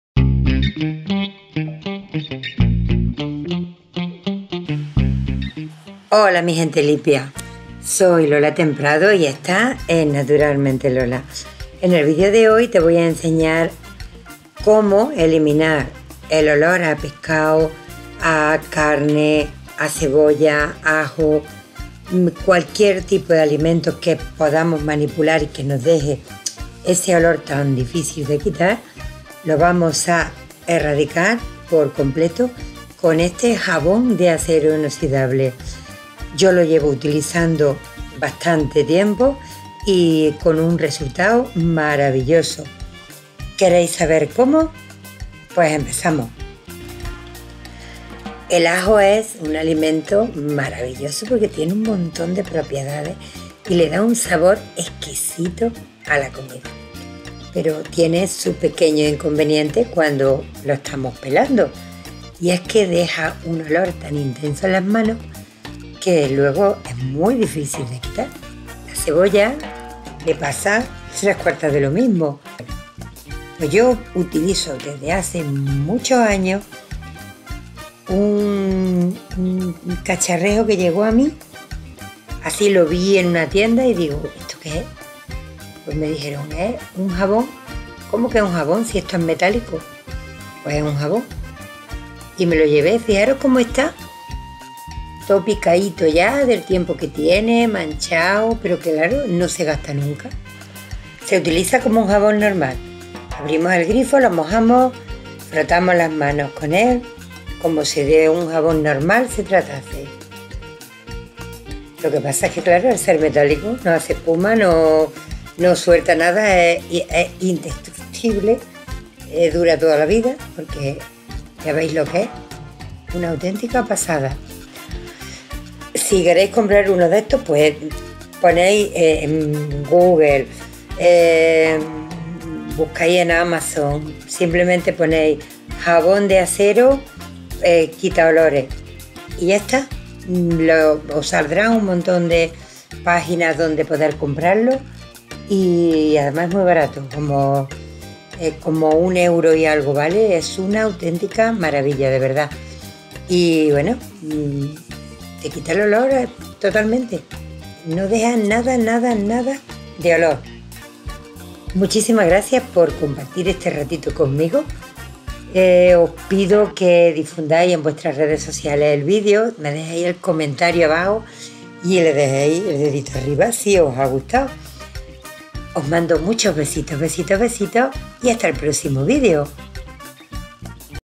ELIMINA EL OLOR A PESCADO ,CEBOLLA, AJO, CARNE DE TUS MANOS USANDO ESTO
Odio el olor a pescado en mis manos, el ajo y la cebolla tampoco me gusta su olor en ellas, asi que os comparto como eliminarlo usando este jabón de acero inoxidable muy barato y eficaz

ESPERO QUE OS GUSTEN IGUAL QUE A MI